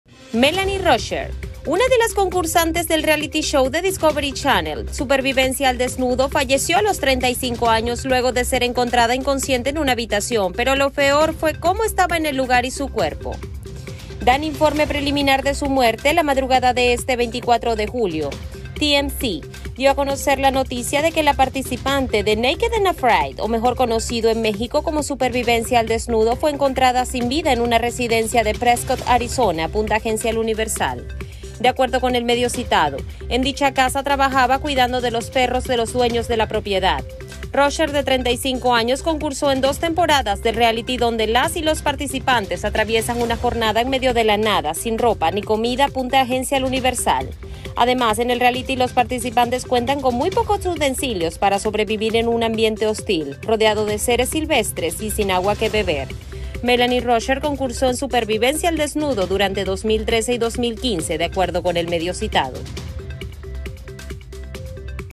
ESTRELLA DE TV MUERTA EN LA CAMA Encuentran a Melanie Rauscher en habitación
Melanie Rauscher, una de las concursantes del reality show de Discovery Channel, “Supervivencia al desnudo”, falleció a los 35 años, luego de ser encontrada inconsciente en una habitación, pero lo pero fue cómo estaba el lugar y su cuerpo. Dan informe preliminar de su muerte.

La madrugada de este 24 de julio, “TMZ” dio a conocer la noticia que la participante de “Naked and Afraid” o mejor conocido en México como “Supervivencia al desnudo” fue encontrada sin vida en una residencia en Prescott, Arizona, apunta Agencia El Universal.